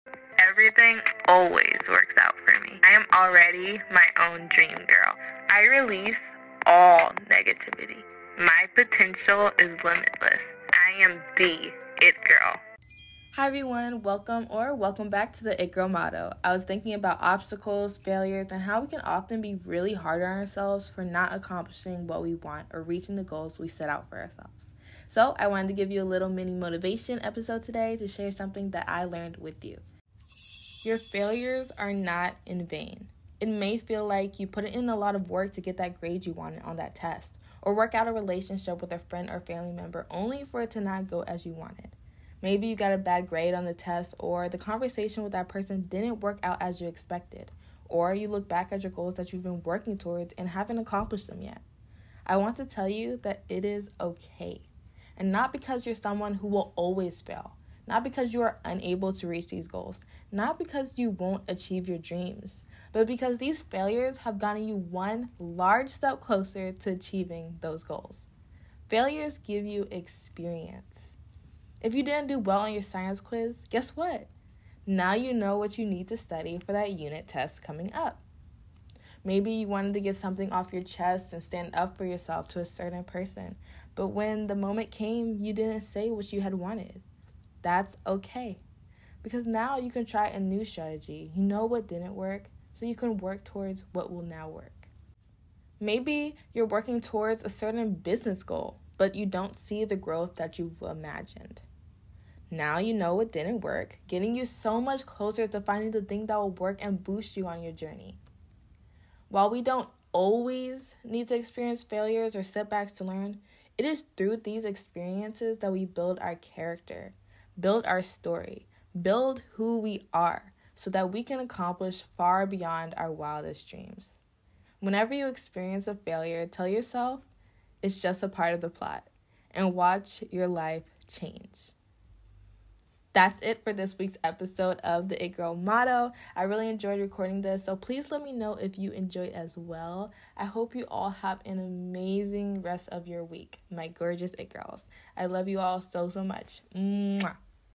The It Girl Motto: Your “Failures” Are Not in Vain, You’re Gaining Something Priceless! Ep.17
Do you need a mini motivation boost? In this episode of The It Girl Motto, Safiyyah shares a lil motivation booster for all the girlies!!

Her Insta: @yayacreatez

Her TikTok: @yayacreatez